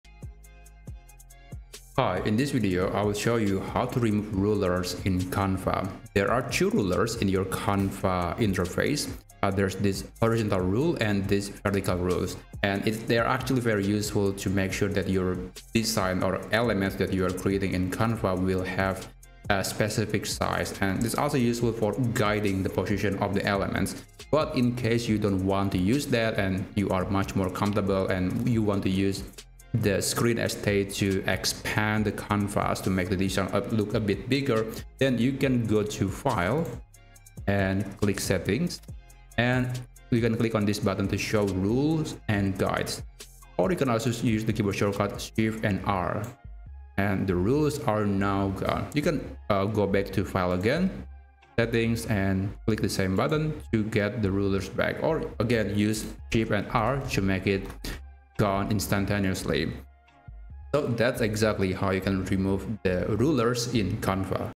How to Remove Rulers in Canva
👉 In this video, learn the quick and easy steps to remove rulers in Canva for a cleaner workspace. Enhance your design experience by maximizing your canvas area without the distraction of gridlines.